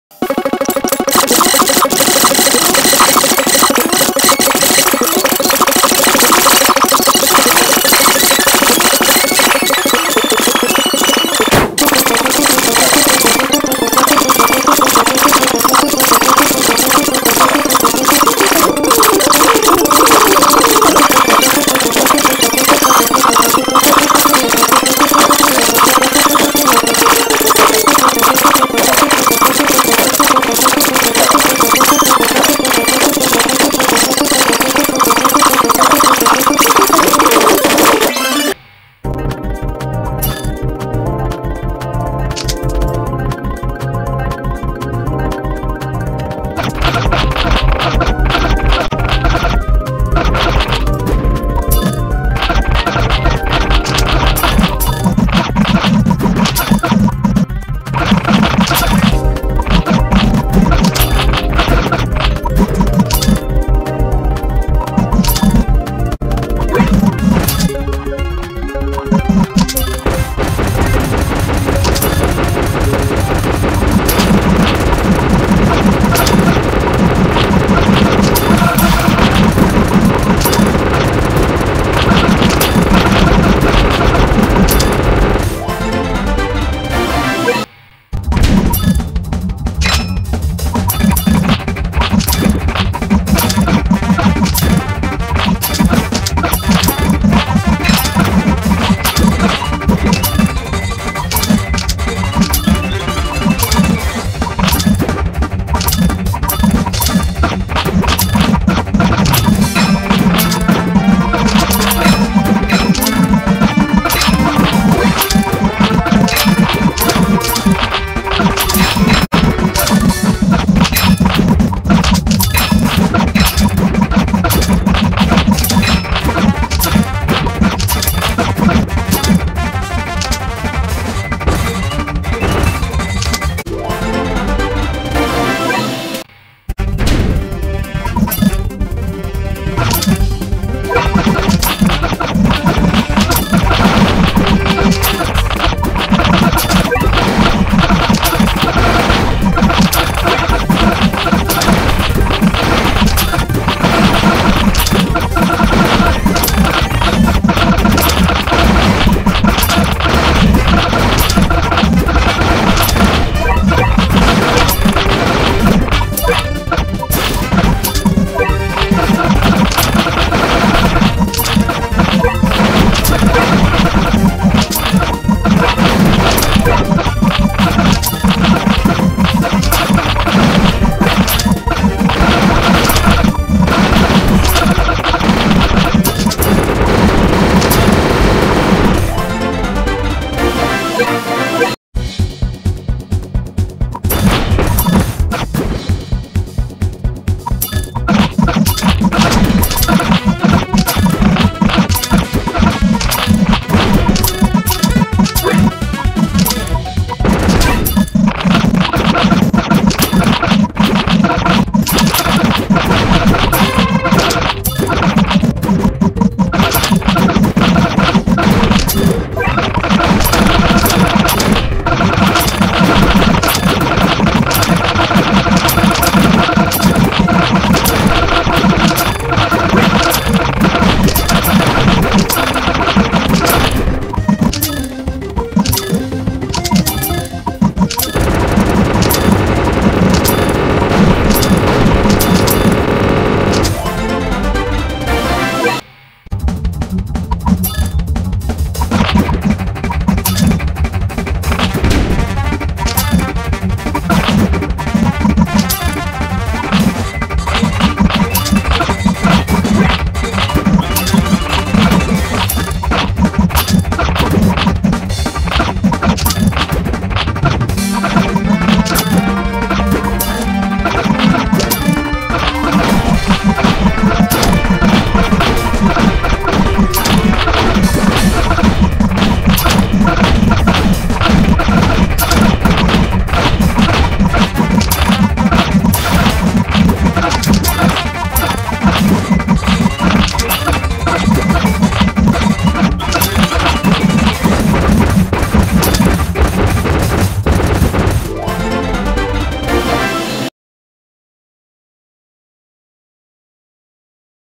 Two unit restriction stories of legend (IT Catacombs)
Really enjoyable and challenging subchapter; easily the hardest two levels were OS Graveyard for needing literal dream level dlazer wave rng, and Soap Scum because of Razorback

Levels:
Haniwa: 50
Cyborg: 50+27 (max survive talent)
Ramen: 50+24 (level 7 defense + level 1 attack talent)
Kalisa: 45
Kyubey: 1
Fishman: 50+7 (max attack talent)
Dark Lazer: 50 (max health + attack + wave + level 8 cost down talent)
Kai: 50
Pizza: 50+11 (max wave + level 5 cost down talent)
Cat Machine: 40 (knockback immune talent)